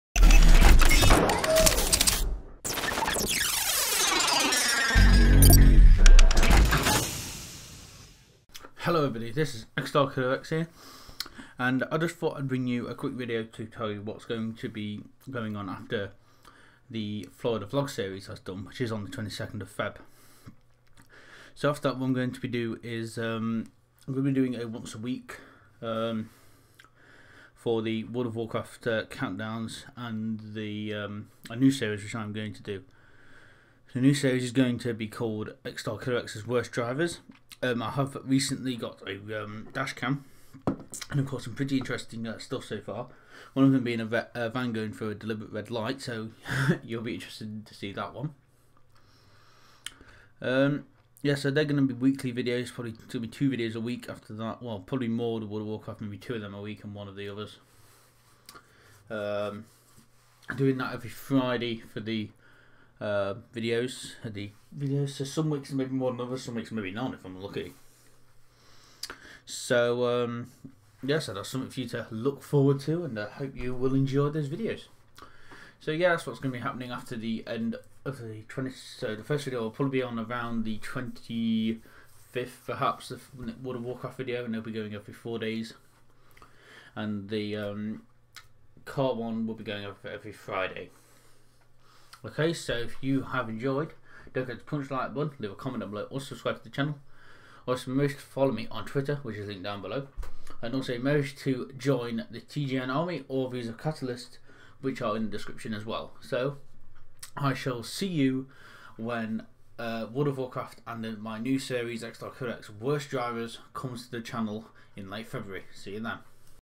XStark1ll3rX Worst Drivers Starting 27th February!
Welcome to XStark1ll3rX's Worst drivers!! Today we will take a look at the worst 4 drivers i saw this week!!! :D